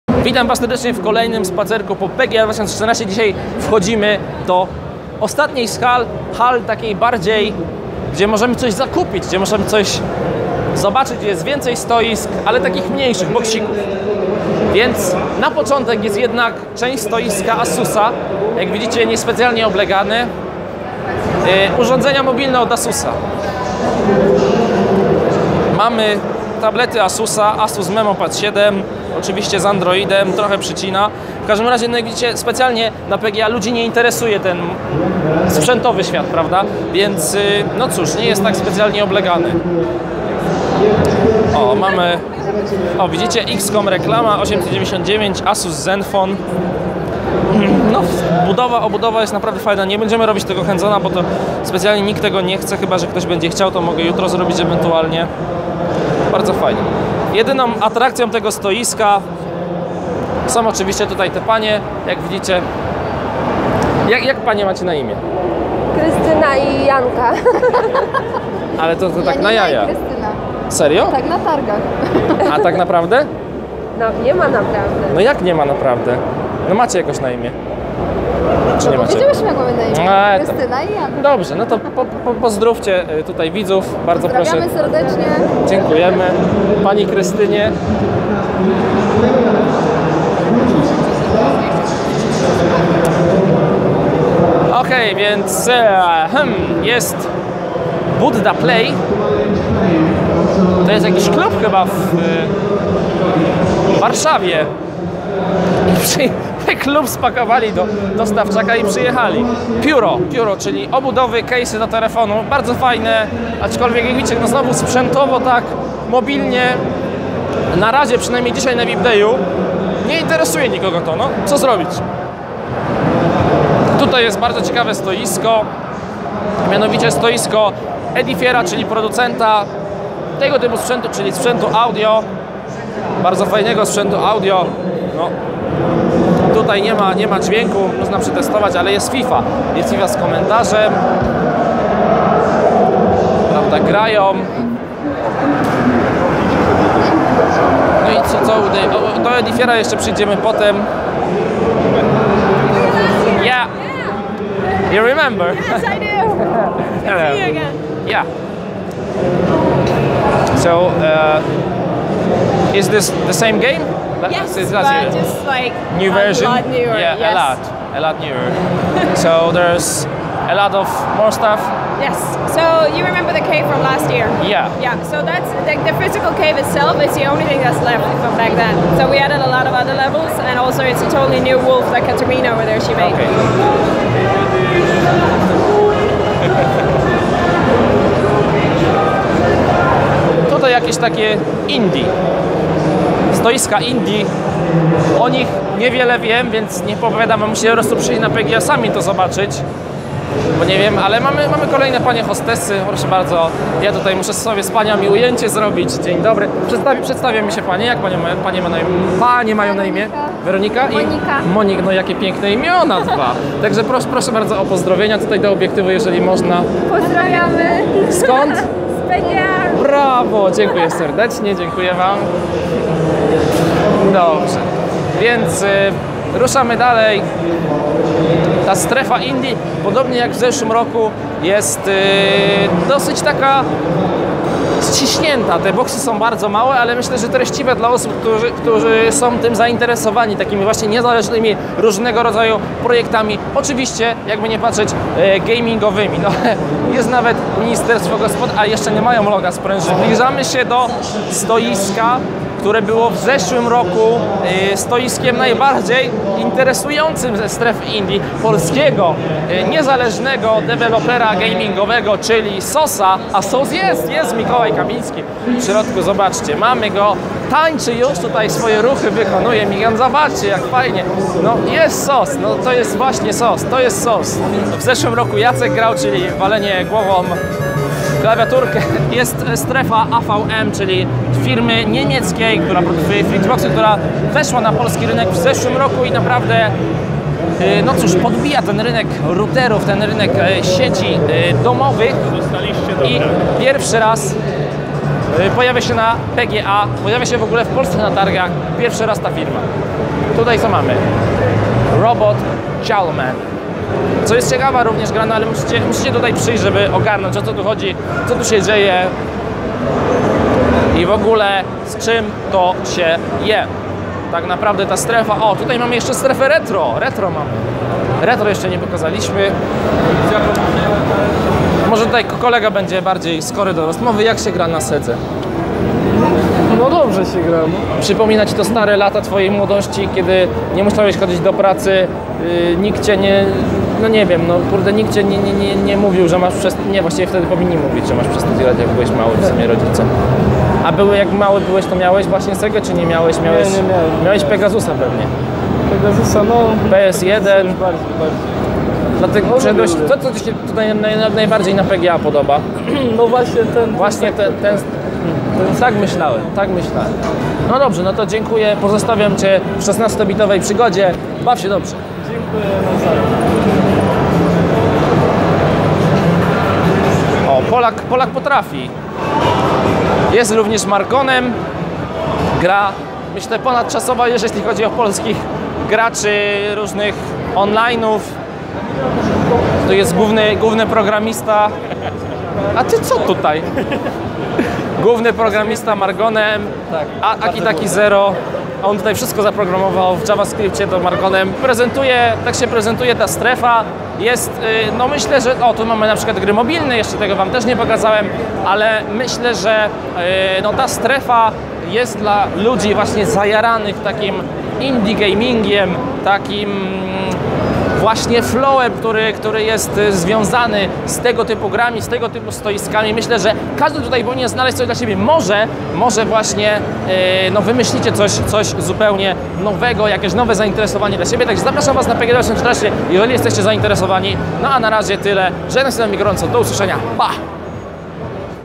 PGA 2014: Spacer po hali 7 (Puro, Strefa INDIE, Oculus Rift, Retro)
Redakcja Portalu
VideoTesty.pl